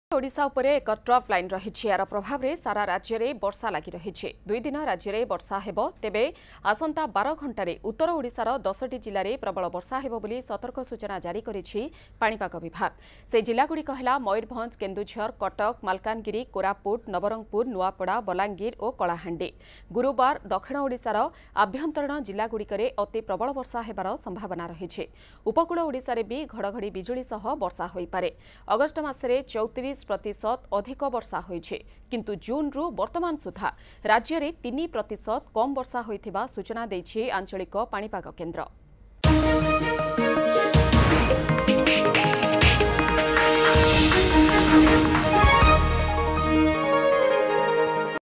Heavy Rainfall And Thunderstorm To Hit Odisha Within Next 12Hours
Heavy Rainfall And Thunderstorm To Hit In Odisha Within Next 12Hours